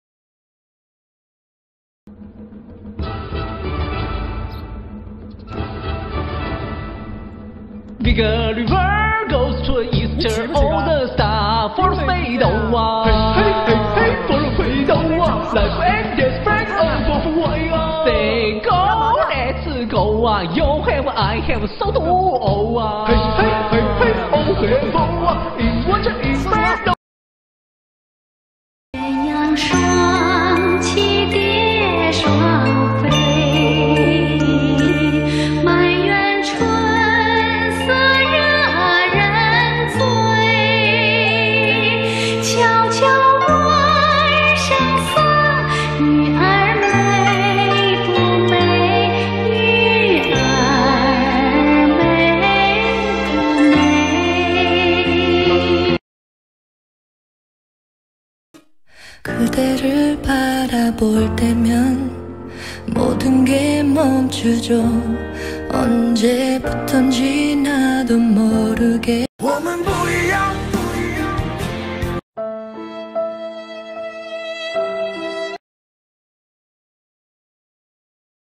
[J4F] Khi ''FAN CÓ TÂM'' (FANTI) làm video =)))))))))))))))))))) |TFBOYS
Mị không phải Fanti, mị chỉ là một bé Tứ Diệp Thảo ngây thơ,trong sáng và bé nhỏ lạc trôi giữa dòng Fanti đông đúc =))
Mà nói đến Fanti thì ở Việt Nam, má Bành ở TFBoys Plus chắc có tâm nhất =)))